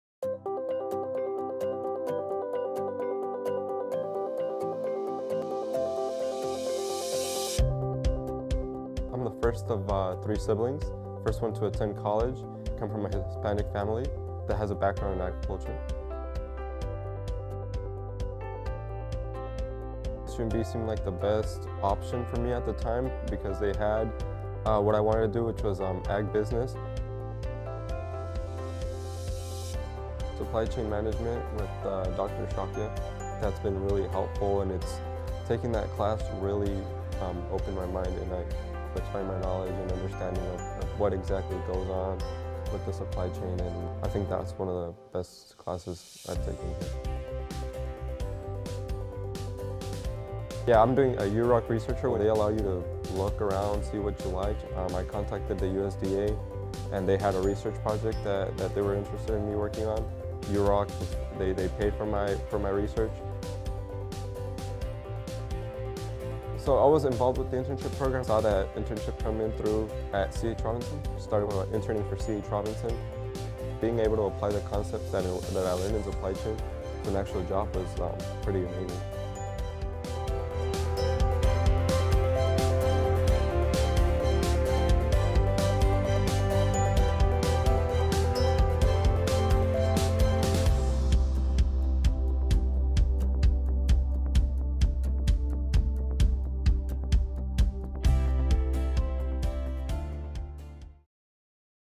Why CSUMB College of Business is the Right Choice - Interview with Roberto Ornelas
Roberto Ornelas talks about his experience at CSUMB's College of Business as a B.S. Business Administration student with an Agribusiness concentration. Highlights include Roberto's USDA research project, facilitated by UROC (Undergraduate Research Opportunity Center), the Supply Chain Management class, and his internship with logistics industry leader, C.H. Robinson.